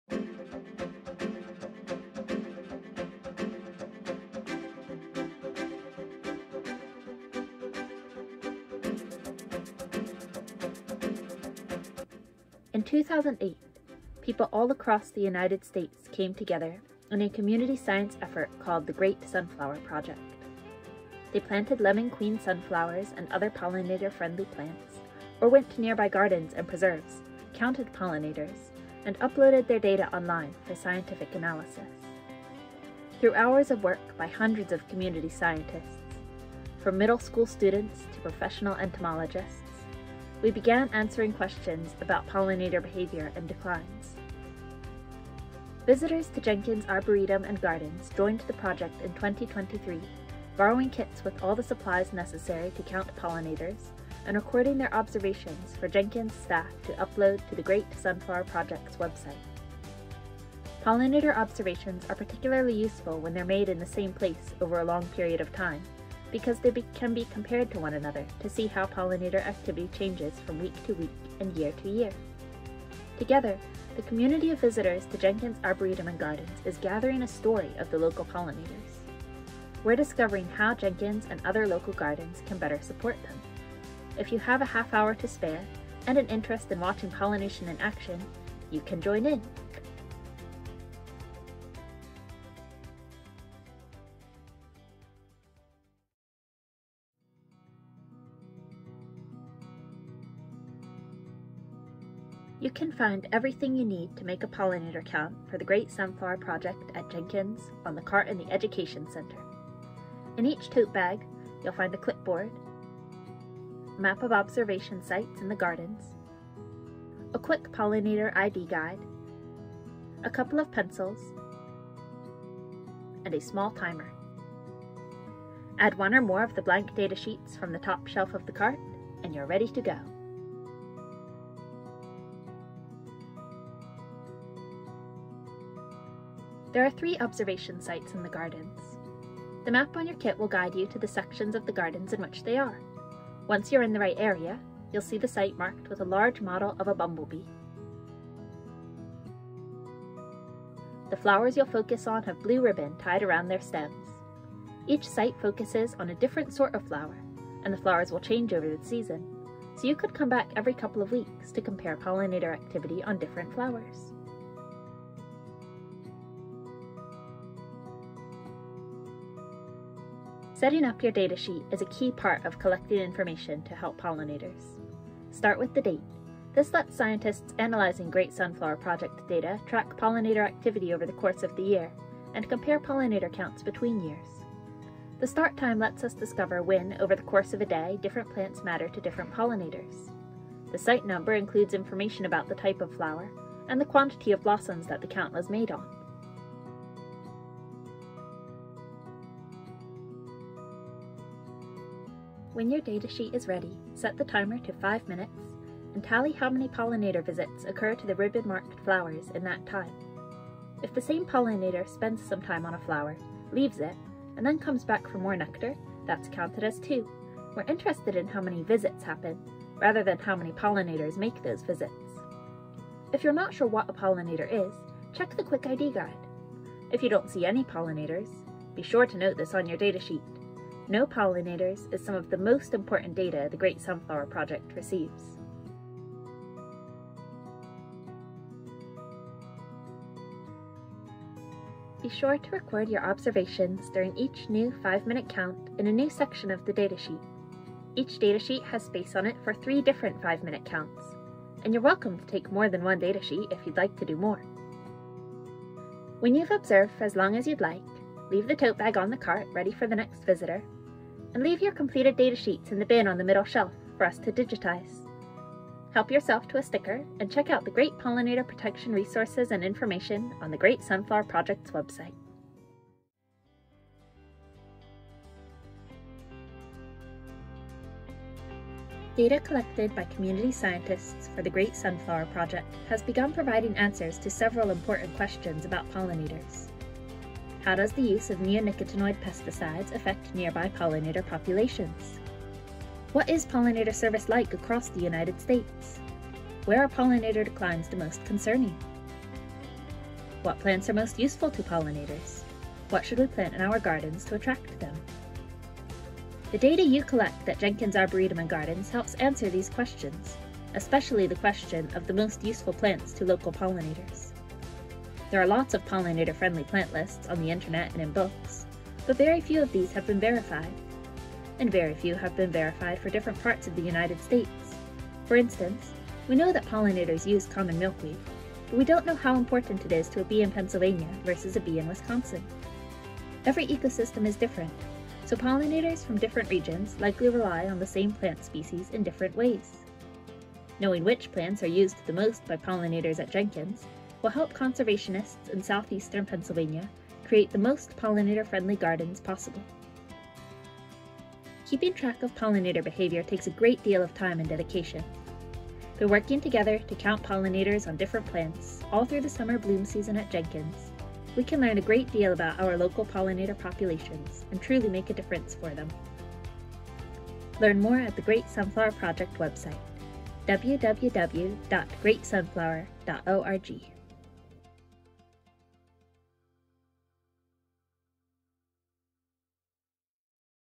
The Great Sunflower Project at Jenkins
Passionate about protecting pollinators? The Great Sunflower Project is a way people across the USA are joining the effort to save them! Jenkins Arboretum & Gardens invites you to borrow a kit and participate in citizen science research in our gardens during the summer months.